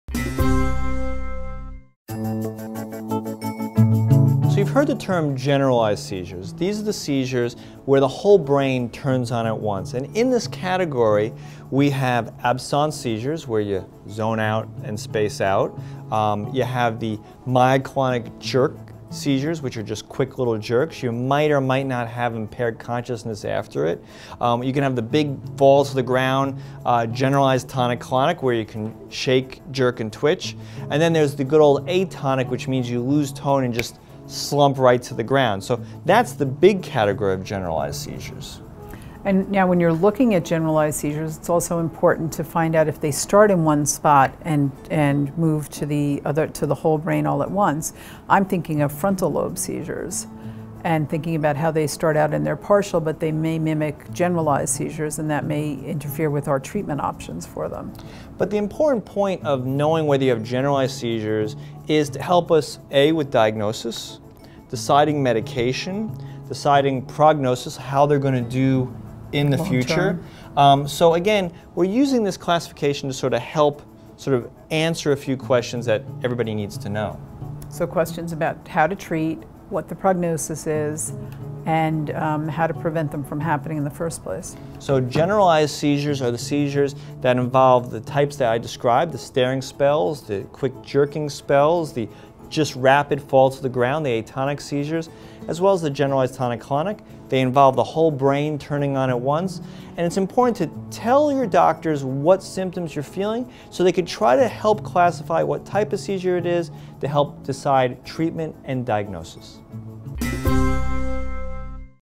What Is a Generalized Seizure? | Epilepsy
So you've heard the term generalized seizures, these are the seizures where the whole brain turns on at once and in this category we have abson seizures where you zone out and space out.  You have the myoclonic jerk seizures which are just quick little jerks you might or might not have impaired consciousness after it.  You can have the big falls to the ground generalized tonic clonic where you can shake jerk and twitch and there's the good old a tonic which means you lose tone and just slump right to the ground, so that's the big category of generalized seizures.

Now, when you're looking at generalized seizures it's also important to find out if they start in one spot and move to the whole brain all at once.  I'm thinking of frontal lobe seizures and thinking about how they start out and their partial but they may mimic generalized seizures and that may interfere with our treatment options for them.  But the important point of knowing whether you have generalized seizures is to help us a. with diagnosis, deciding medication, deciding prognosis how they're going to do in the future so again we're using this classification to sort of help answer a few questions that everybody needs to know.  So questions about how to treat, what the prognosis is, and how to prevent them from happening in the first place.

So generalized seizures are seizures that involve the types that I described, the staring spells, the quick jerking spells, the rapid fall to the ground the atomic seizures, as well as the generalized tonic clonic they involve the whole brain turning on at once and it's important to tell your doctors what symptoms you're feeling so they can try to help classify what type of seizure it is to help decide treatment and diagnosis.